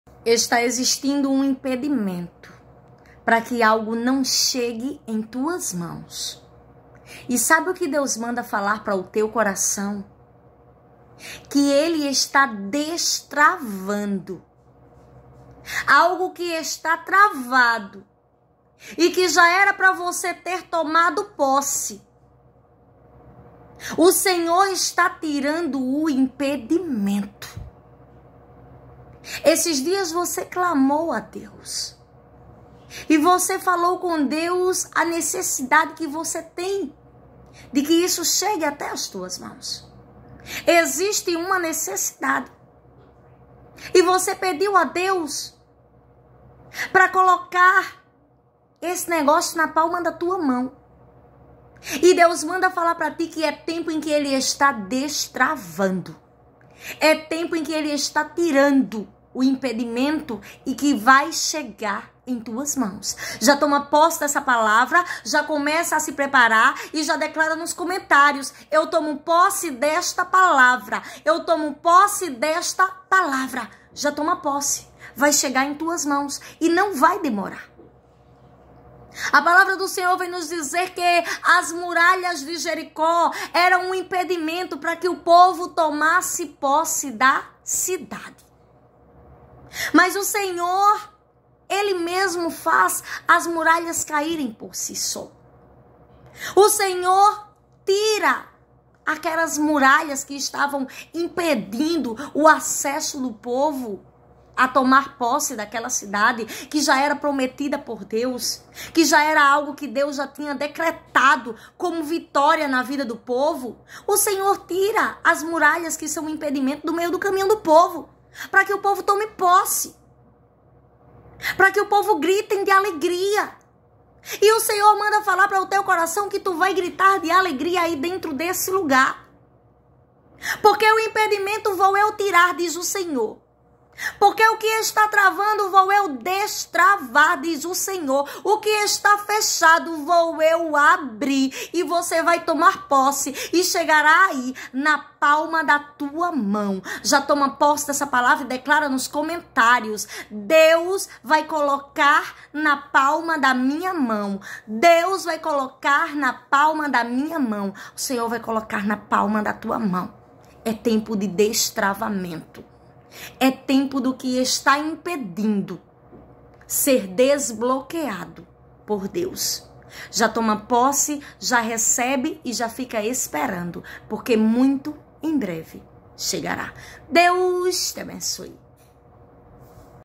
Deus vai destravar algo pra chegar em tuas mãos!!!
Deus está destravando uma situação pra a vitória chegar em tua mão.